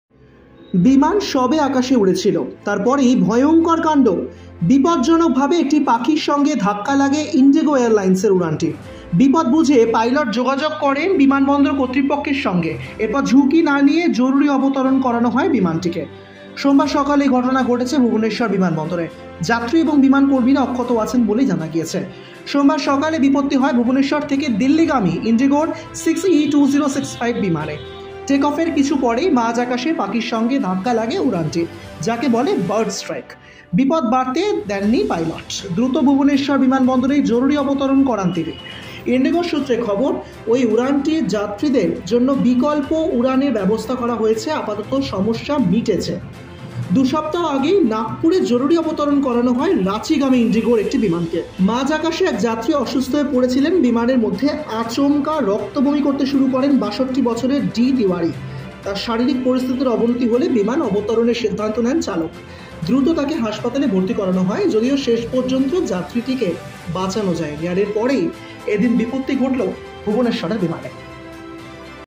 National News: মাঝ আকাশে আচমকাই ঘটল বিপত্তি, ভেঙে পড়ার আগেই রক্ষা পেল বিমান | Bangla News
National News: মাঝ আকাশে আচমকাই ঘটল বিপত্তি, ভেঙে পড়ার আগেই রক্ষা বিমানের। বিমান সবে আকাশে উড়েছিল। তার পরেই ভয়ংকর কাণ্ড। বিপজ্জনকভাবে একটি পাখির সঙ্গে ধাক্কা লাগে ইন্ডিগো এয়ারলাইন্স উড়ানটির। বিপদ বুঝে পাইলট যোগাযোগ করেন বিমানবন্দর কর্তৃপক্ষের সঙ্গে। এর পর ঝুঁকি না নিয়ে জরুরি অবতরণ করানো হয় বিমানটিকে। | Bangla News